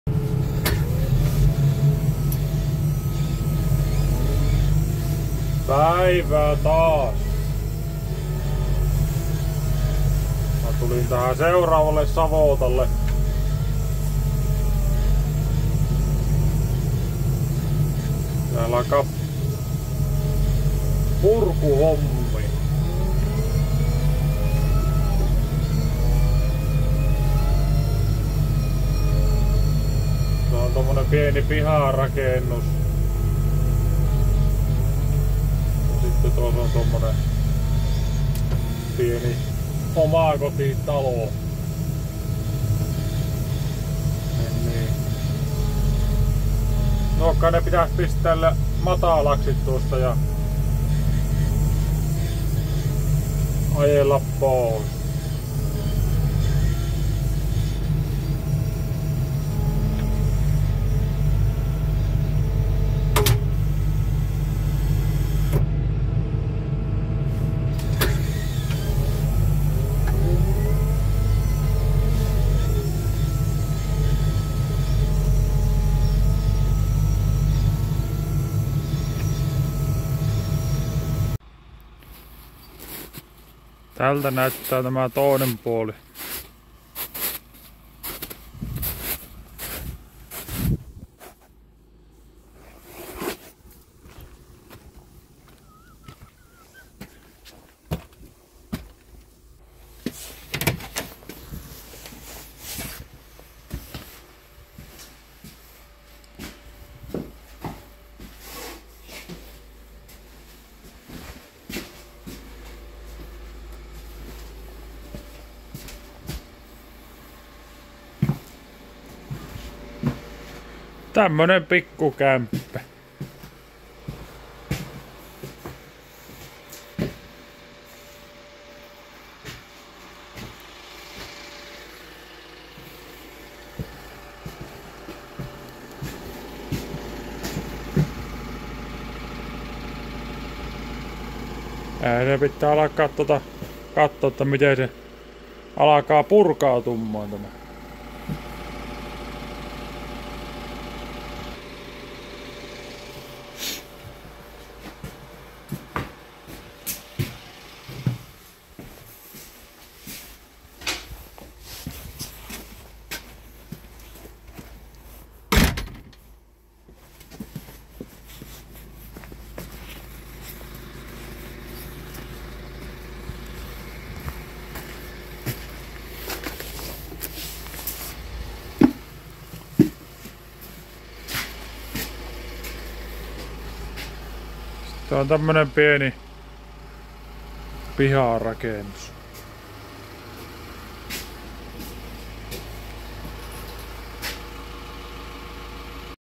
Arriving at the demolition site. purkutyömaalle saapuminen. volvo ewr150E+engcon+trimble
tämmöne savotta. pitäs purkaa nämä rakennukset pois.

such a construction site. I need to demolish these buildings